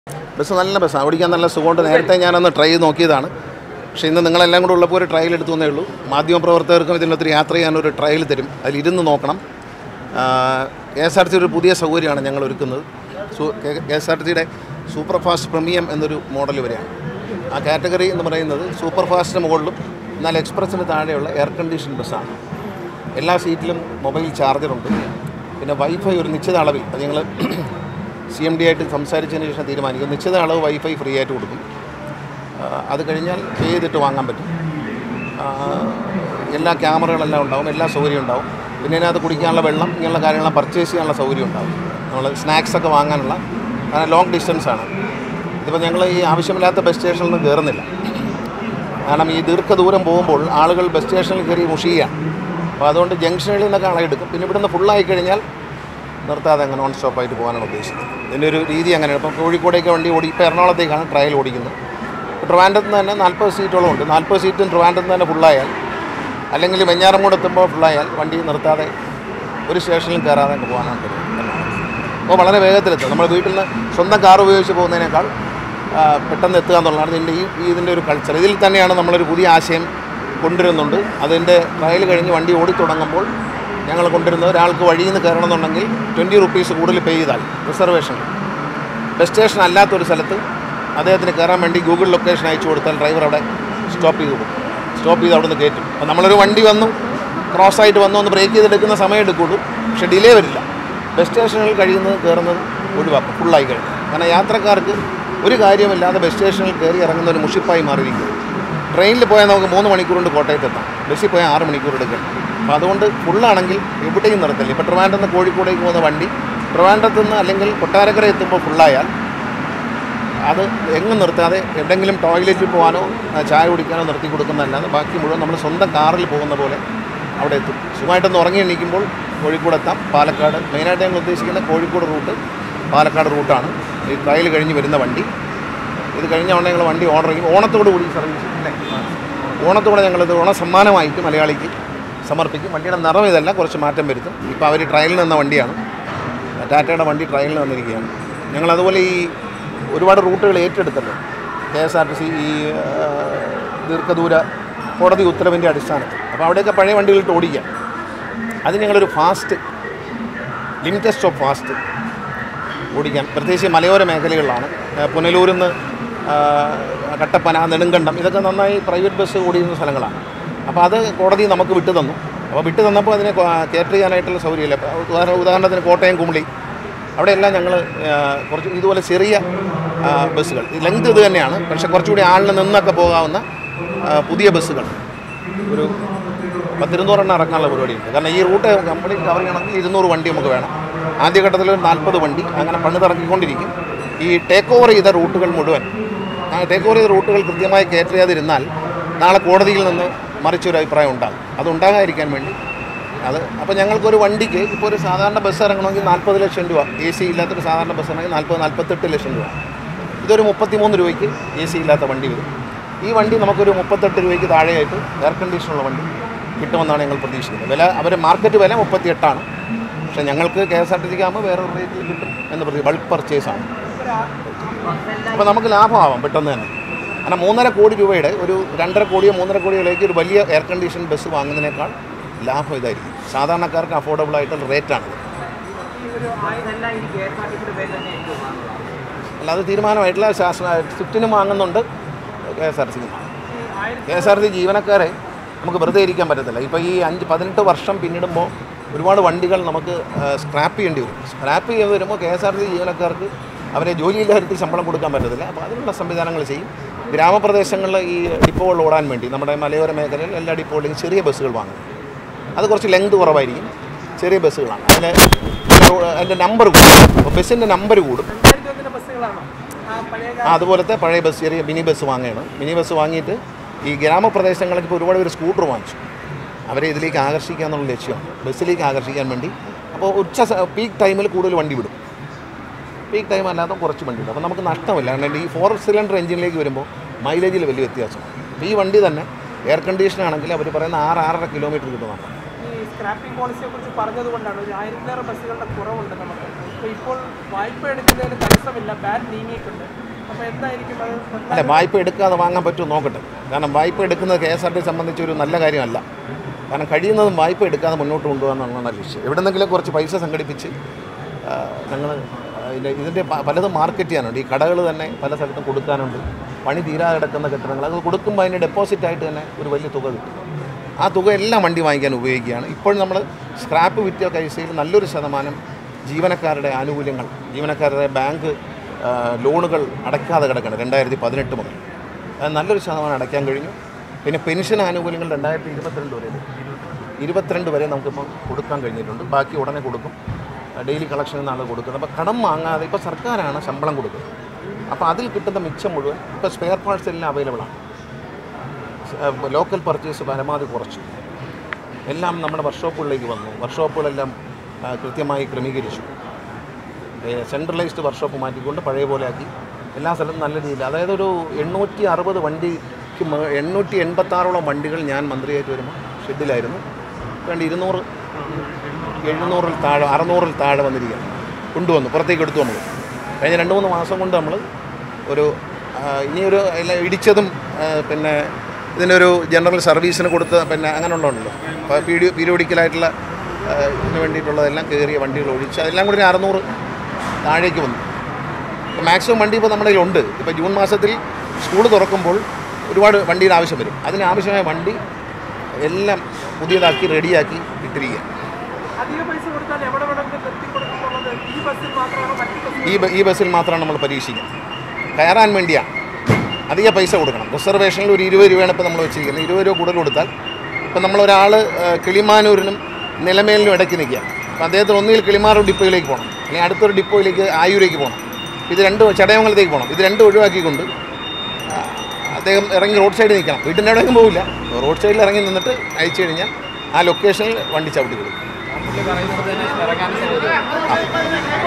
'സ്‌നാക്‌സ്, വെള്ളം എല്ലാം ബസിൽ തന്നെ വാങ്ങാം, ഒപ്പം വൈഫൈയും' | KB Ganeshkumar | KSRTC
'സ്‌നാക്‌സ്, വെള്ളം എല്ലാം ബസിൽ തന്നെ വാങ്ങാം, ഒപ്പം വൈഫൈയും'; KSRTC പ്രീമിയം സൂപ്പർഫാസ്റ്റ് ബസിന്റെ ട്രയൽ റൺ നടത്തി മന്ത്രി ഗണേഷ് കുമാർ

Happy viewing!